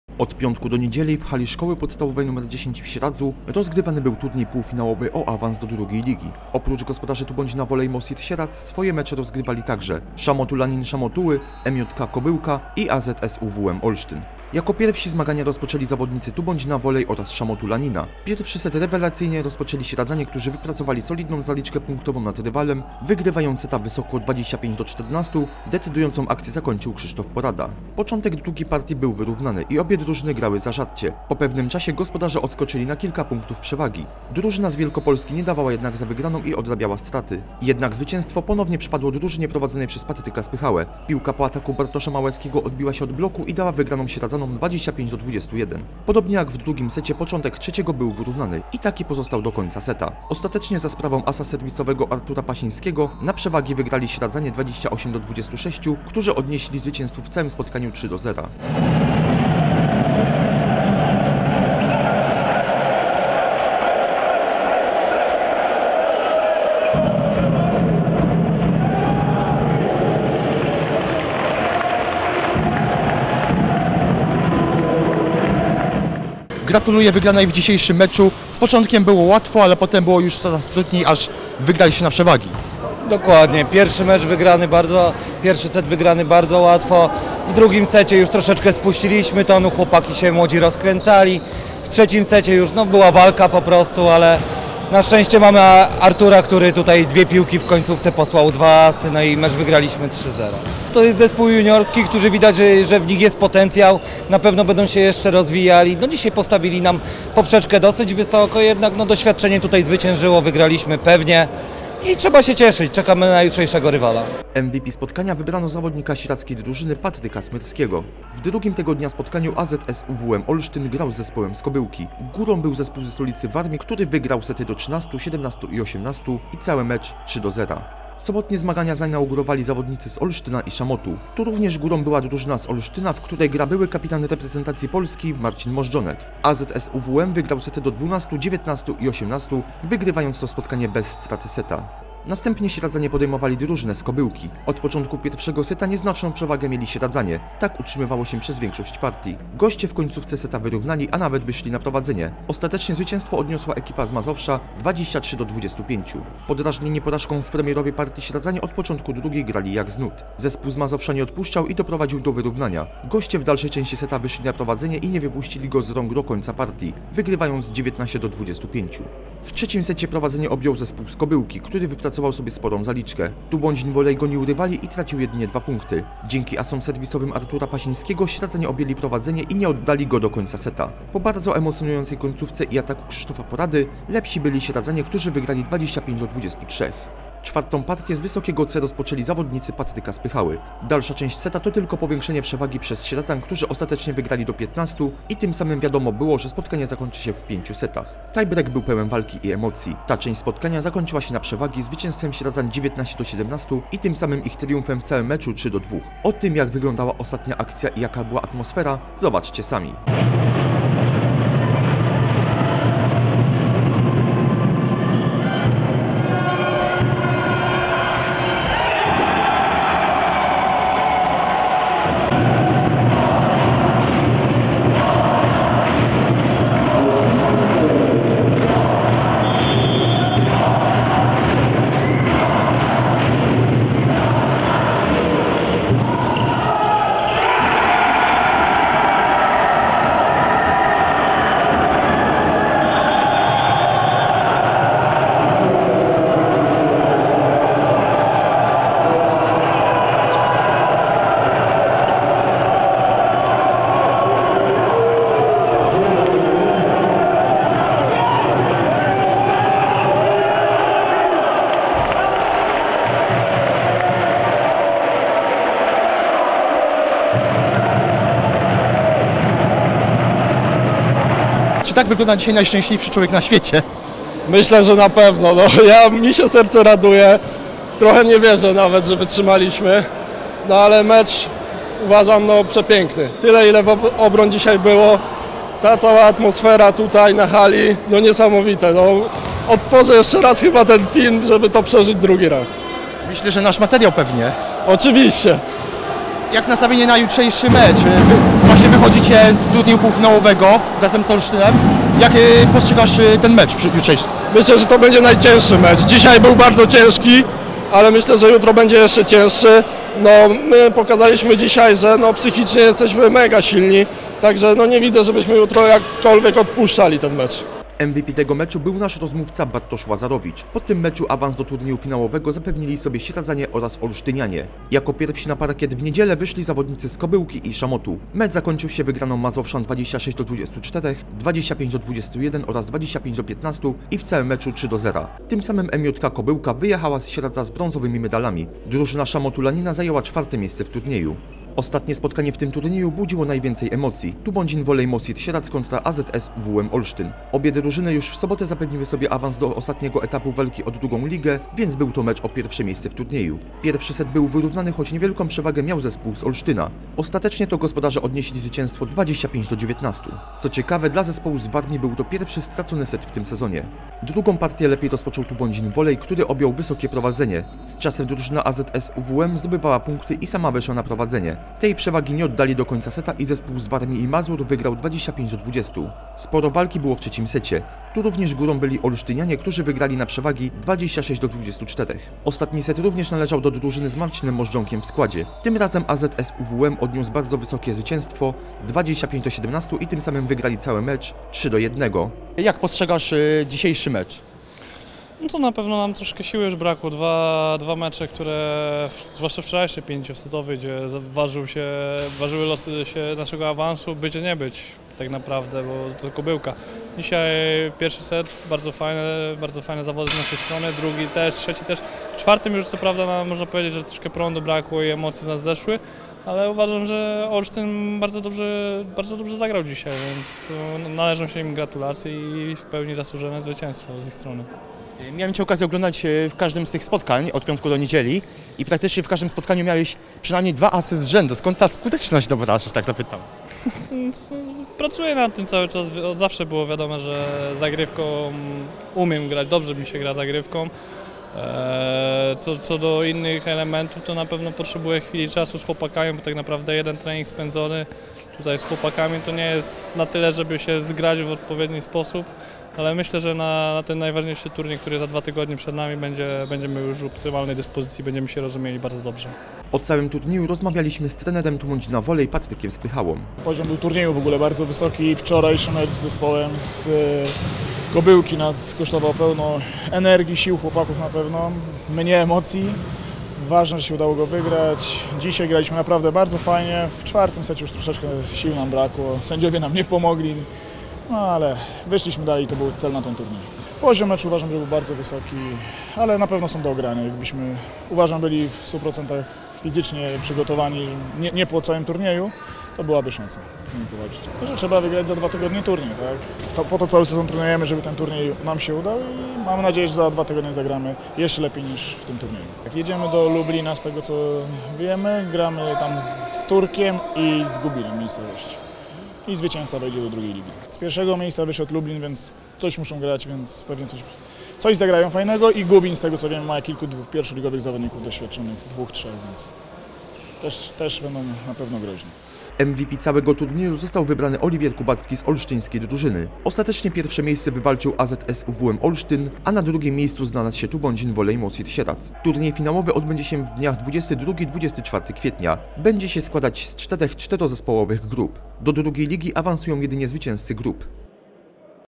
Turniej półfinałowy o awans do II ligi
Za nami drugi z trzech etapów walki o II ligę siatkówki. W miniony weekend w Sieradzu odbył się turniej półfinałowym. Na trybunach był doping, a na boisku walka i emocje. Tubądzin Volley awansował do ostatniej fazy, a na parkiecie oglądaliśmy byłego kapitana reprezentacji Polski.